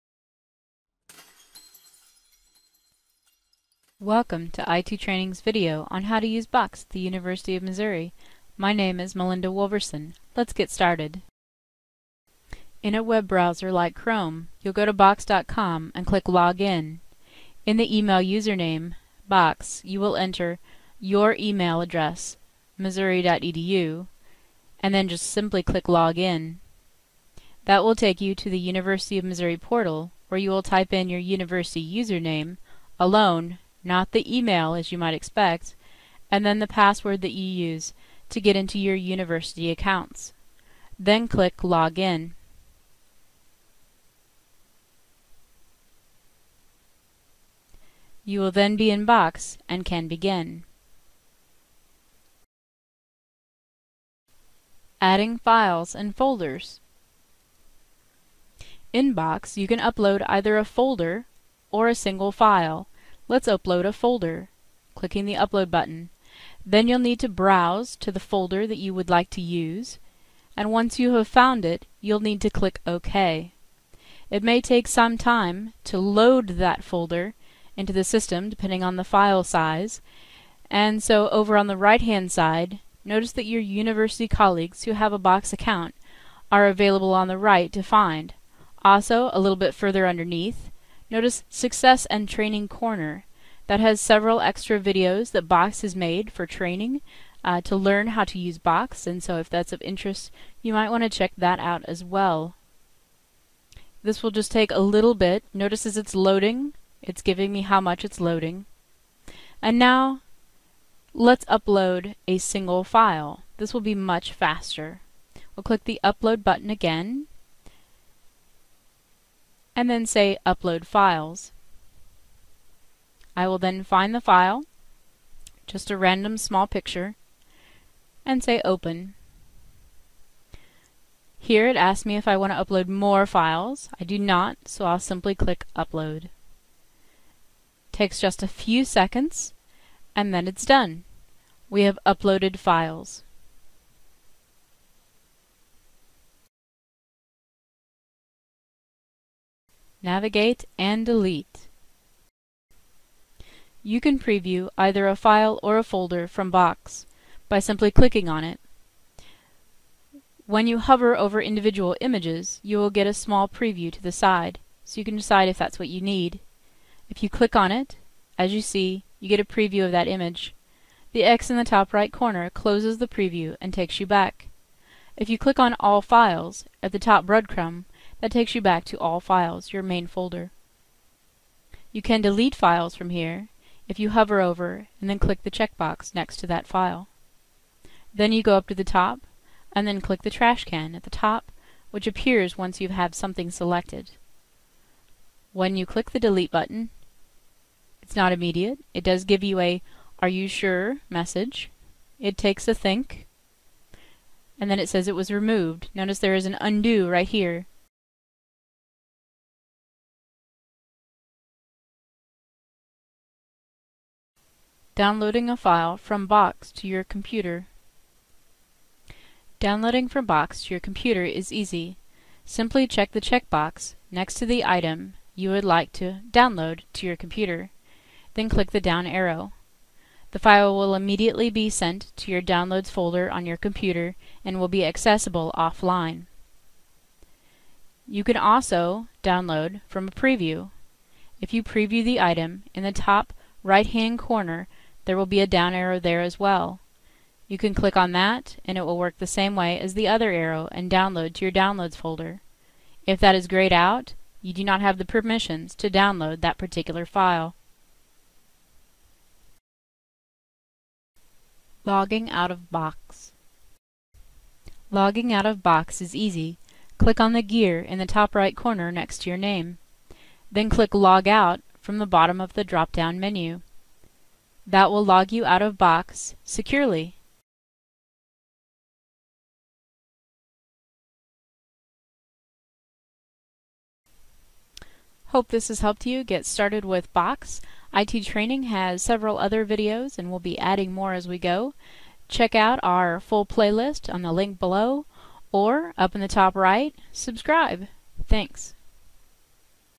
Box: Getting Started at Mizzou
Box is an online storage solution, approved by the University of Missouri-Columbia for campus use. Emails with the @umsystem.edu extension will work for box accounts as well. Video includes: university login, adding files and folders from your computer, basic navigation, deleting files, downloading files to your computer from Box, and logging out.

Video by: -Mel. Wolverson